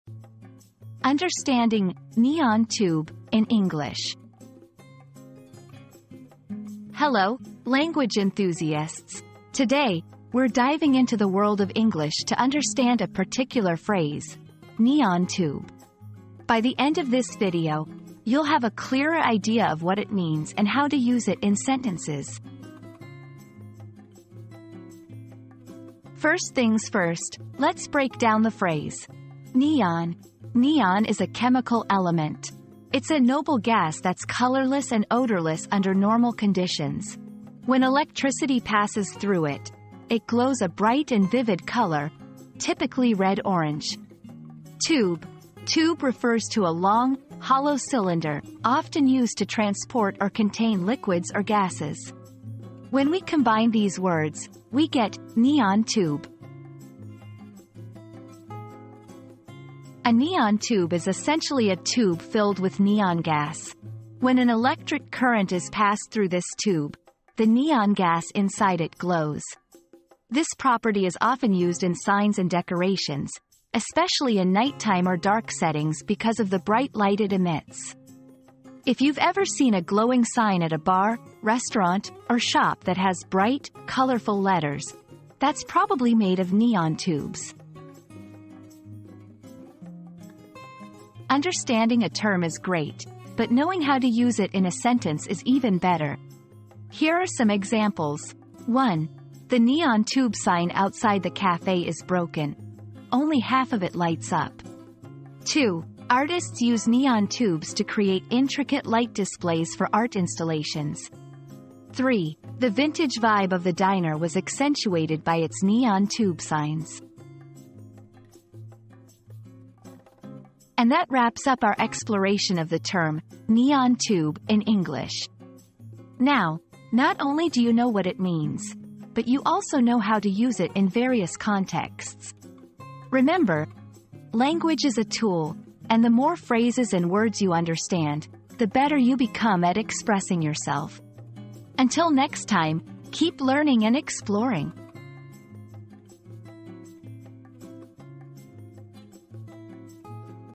Understanding "Neon Tube" in English
Unveiling the Brilliance of Neon Tubes • Discover the fascinating world of neon tubes in this engaging video. From understanding the components to exploring its usage in sentences, you'll gain a comprehensive understanding of this luminous phenomenon.

00:00 • Introduction - Understanding "Neon Tube" in English
00:25 • Breaking Down the Words
00:59 • What is a Neon Tube?
01:30 • Usage in Sentences